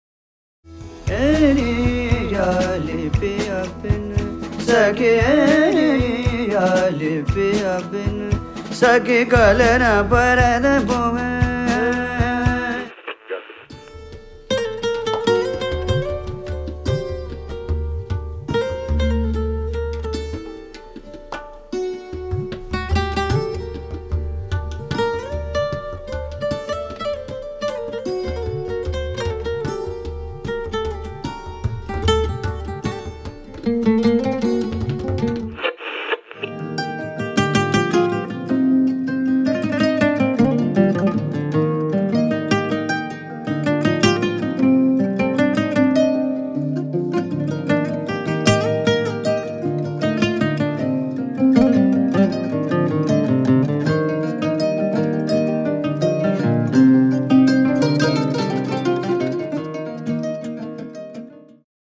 Giuliano Modarelli - promo
Taking you on a journey through India and Europe, guitarist Giuliano Modarelli is one of the most interesting musicians to emerge from the world music scene over the last few years. His strong compositional style, studious attitude and unique approach to the acoustic guitar through the application of sarod technique has led to collaborations with great masters of Hindustani music as well as composing for theatre, dance and cinema.